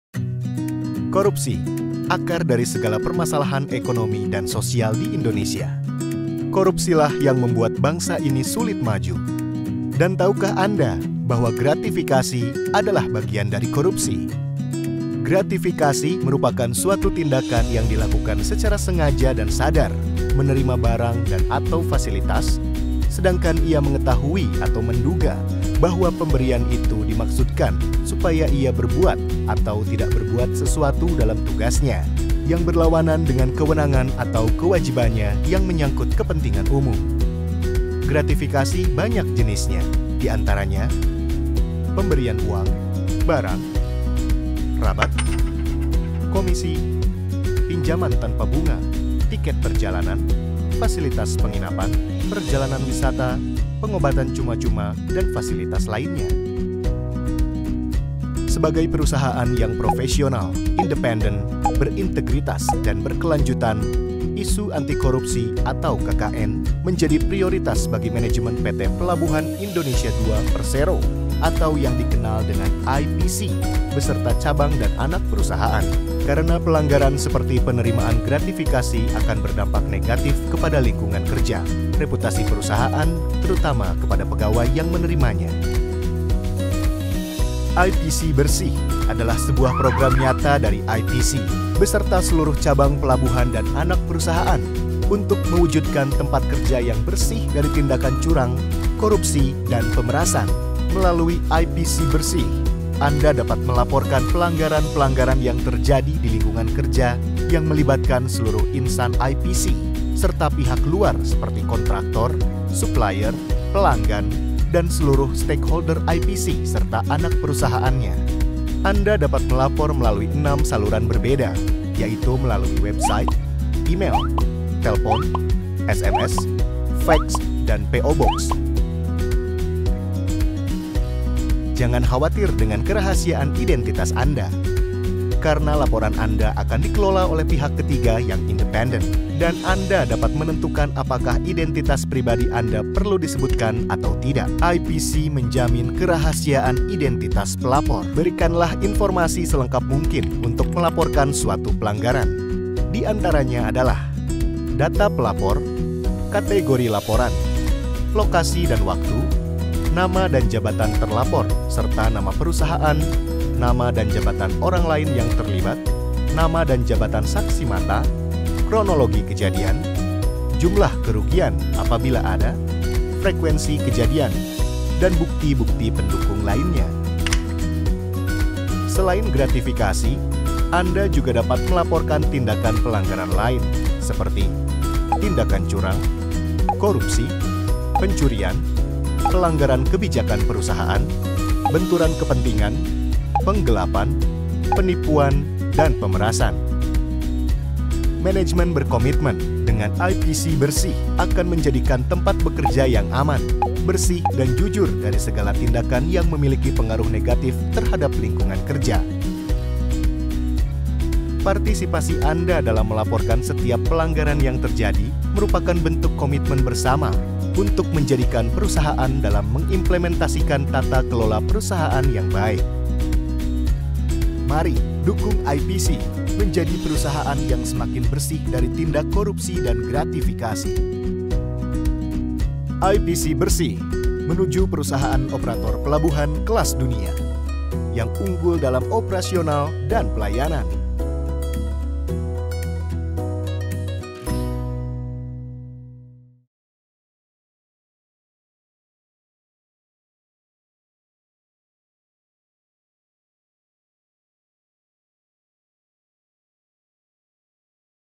Whistle Blowing System (WBS) IPC
IPC Bersih adalah sebuah program nyata dari manajemen PT. Pelabuhan Indonesia II (Persero) (atau disebut juga Indonesia Port Corporation selanjutnya disingkat IPC) beserta seluruh cabang pelabuhan dan Anak Perusahaan untuk mewujudkan tempat kerja yang bersih dari tindakan curang, korupsi dan pemerasan.

Salah satu aplikasi yang disediakan oleh PT. Pelabuhan Indonesia II (Persero) bagi seluruh stakeholder yang memiliki informasi dan ingin melaporkan suatu perbuatan berindikasi pelanggaran yang dilakukan oleh Insan PT. Pelabuhan Indonesia II (Persero) dan Anak Perusahaan adalah Whistleblowing System yang independen. Melalui sistem ini, IPC dapat menjadi perseroan yang lebih baik dan mempunyai daya saing baik di tingkat nasional dan internasional. Bagi Insan IPC atau stakeholder yang melaporkan akan diberikan perlindungan, baik dalam hal kerahasiaan identitas maupun dari kemungkinan tindakan balasan oleh si terlapor.